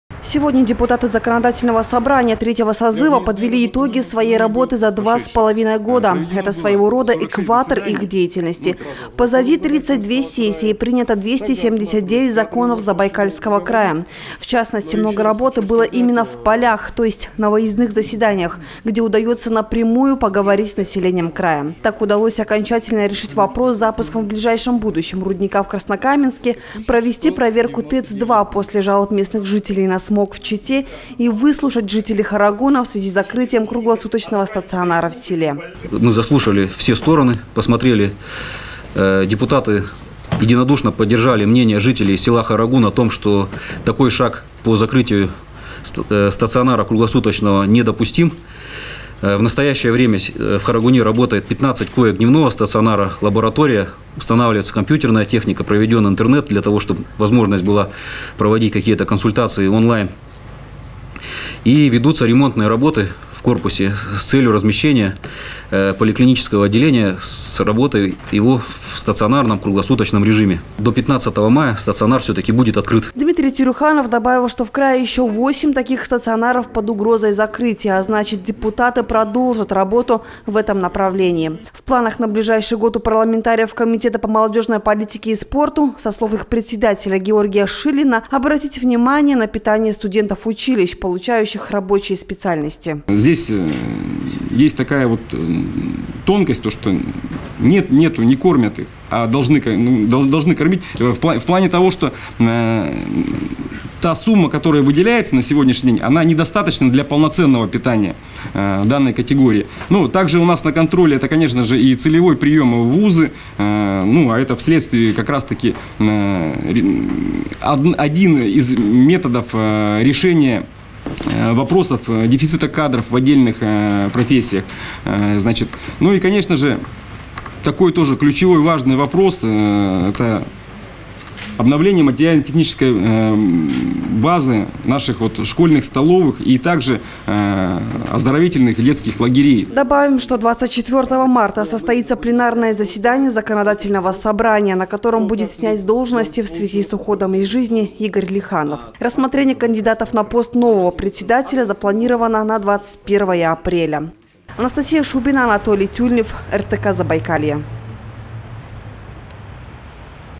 Золотая середина
Сегодня депутаты законодательного собрания третьего созыва подвели итоги своей работы за 2,5 года. Это своего рода золотая середина их деятельности. О том, какие встречи и выезды по районам стали решающими в формировании повесток заседаний, как им удалось решить вопрос со стационаром в Харагуне и чем закончилась проверка ТЭЦ-2 из-за жалоб на смог в Чите, депутаты рассказали сегодня журналистам местных СМИ.